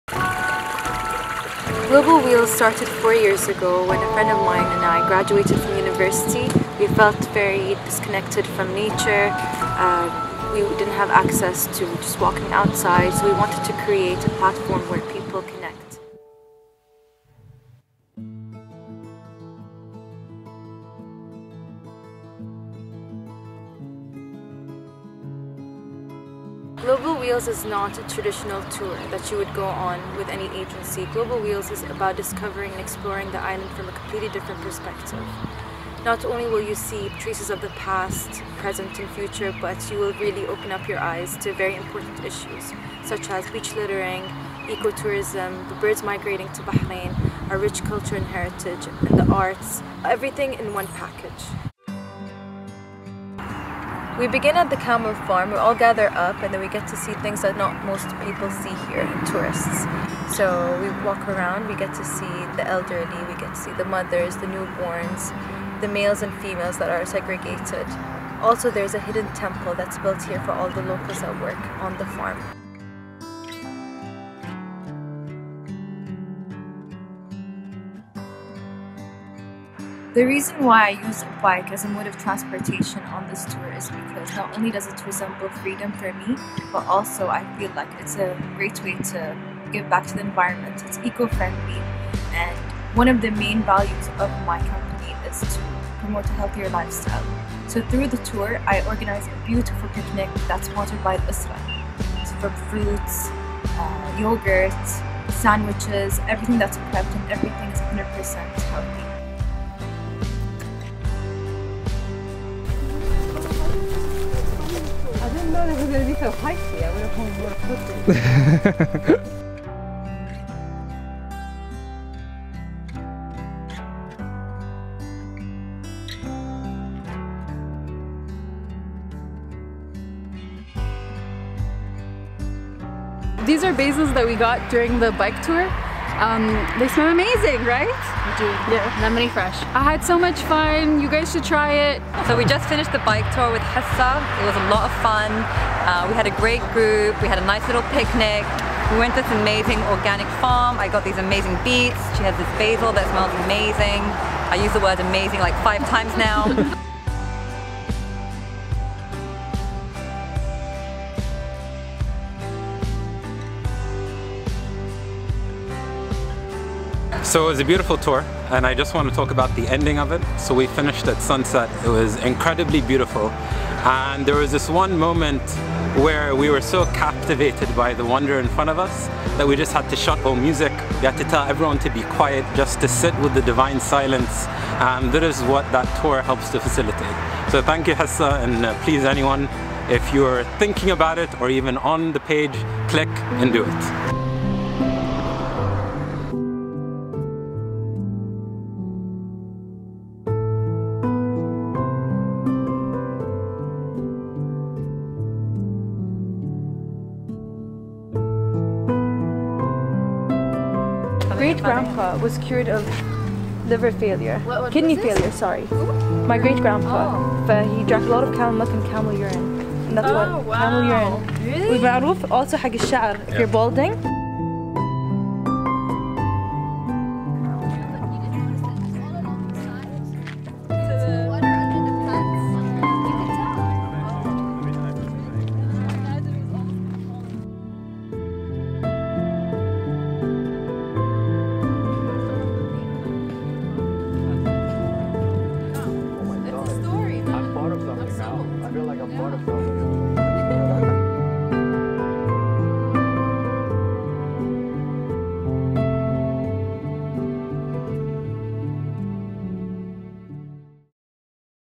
Discover Bahrain's hidden treasures | Global Wheels
This is one of the most incredible tours that I've been on. It's something everyone on this island should experience first hand. The tour is given by Hessa.

Here's a little about Hessa:
Hessa Al Rumaihi is an entrepreneur focused on promoting active living via her cycling tours which allow locals and tourists to experience Bahrain from a unique perspective. Hessa’s vision is to promote a healthier lifestyle, raise awareness about beach littering and the environment (eco tourism) & educate about cultural and historical sites on the island.